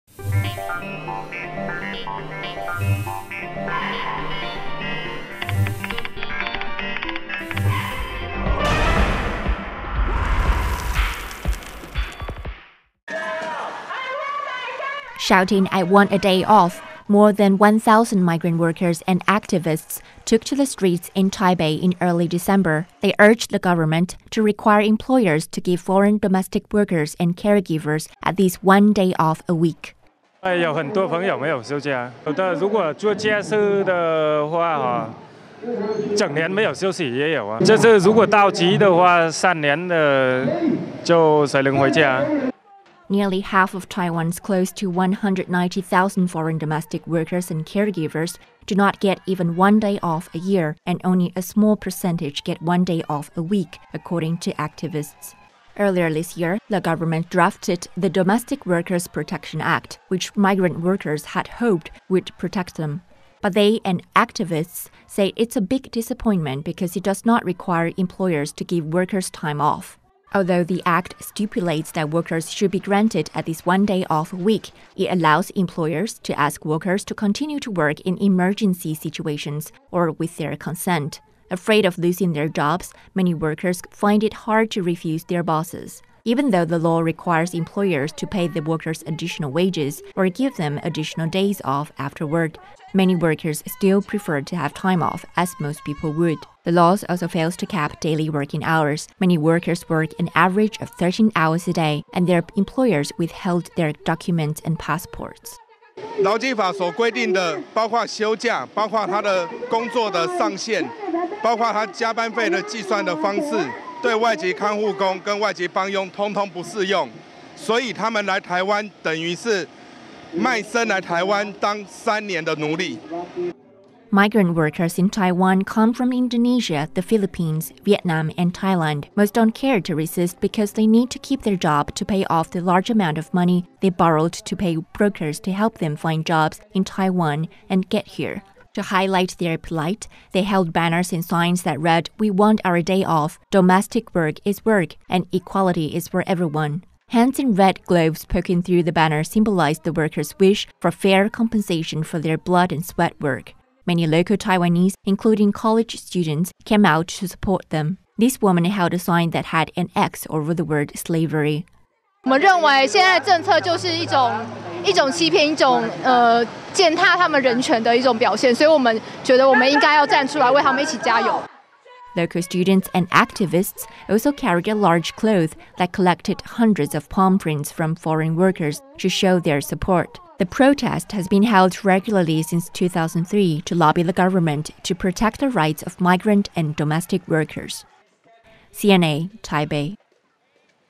Migrant workers demand day off
Shouting "I want a day off," more than 1,000 migrant workers and activists took to the streets in Taipei in early December. They urged the government to require employers to give foreign domestic workers and caregivers at least one day off a week.

Nearly half of Taiwan's close to 190,000 foreign domestic workers and caregivers do not get even one day off a year and only a small percentage get one day off a week, according to activists.